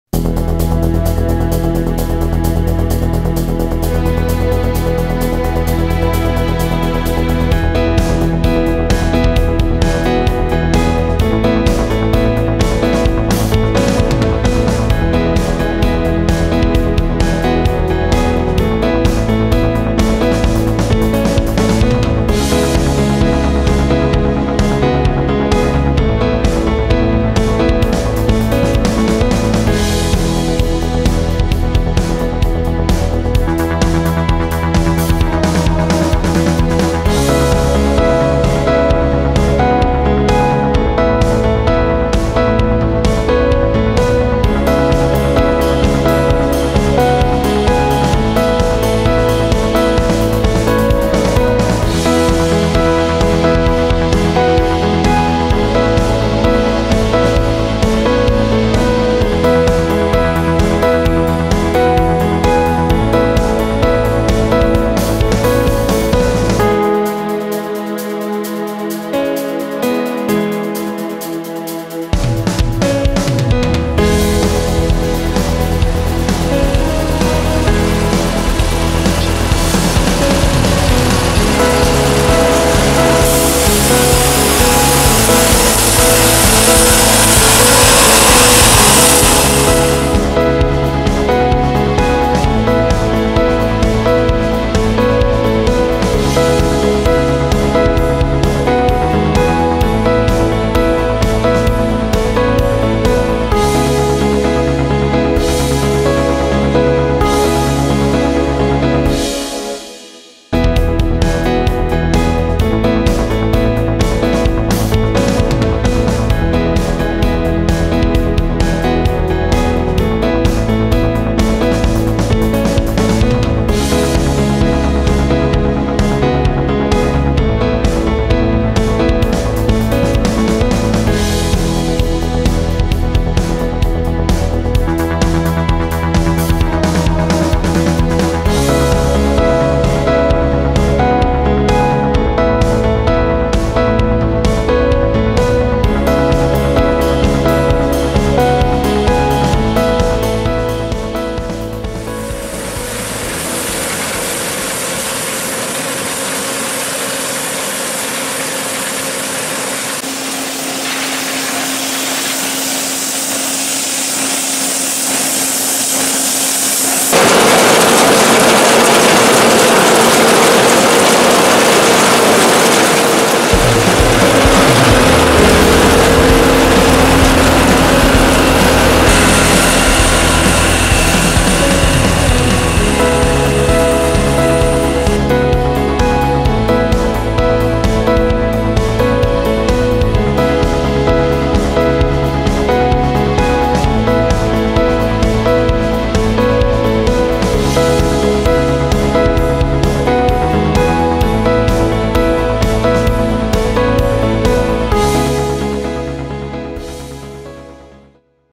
HARVEST CANABIS / SKLIZEŇ KONOPÍ - John Deere 2266 Extra
Sklizeň konopí provádí kombajn JD 2266 Extra.